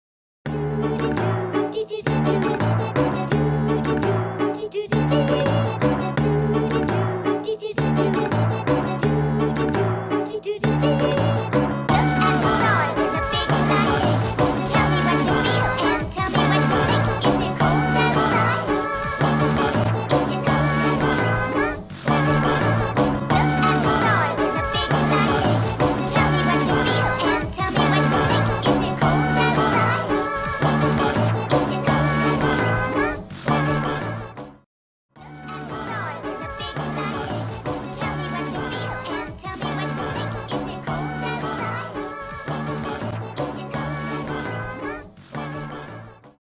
Is it Cold Outside? meme//StarPad oc lore?! 🤩🤩😋
guys this is my first time doing (mostly) traditional animation and also my first time using ToonSquid how did i do?